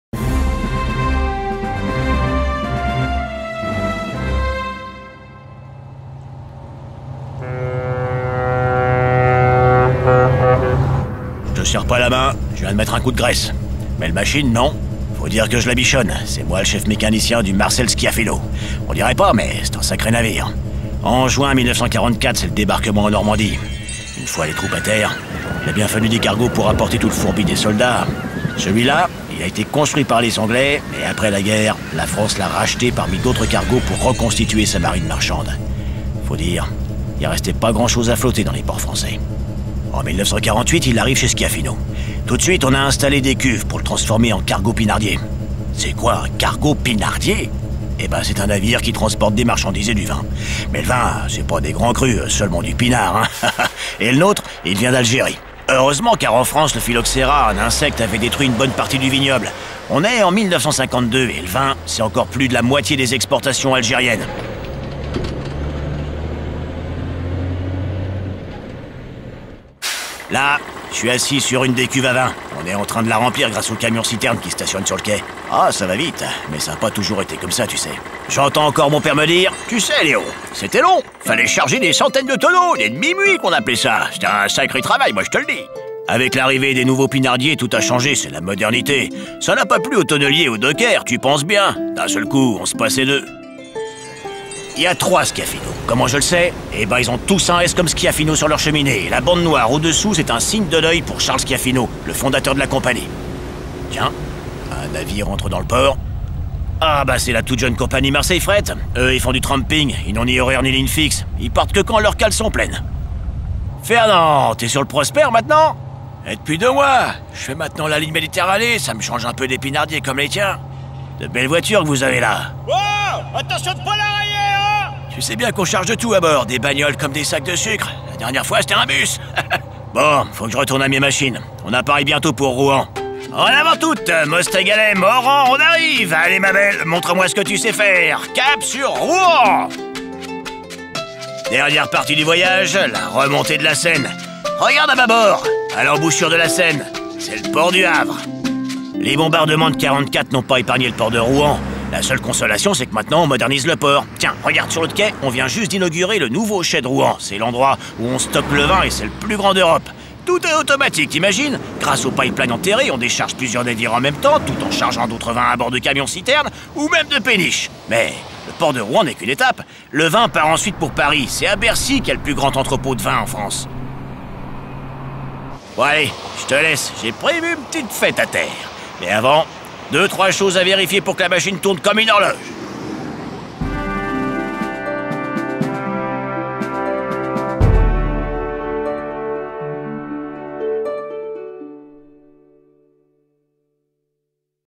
De l'amphore au conteneur / à bord d'un cargo pinardier
Animation réalisée dans le cadre de l'exposition de l' Amphore au conteneur 15 octobre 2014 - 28 juin 2015.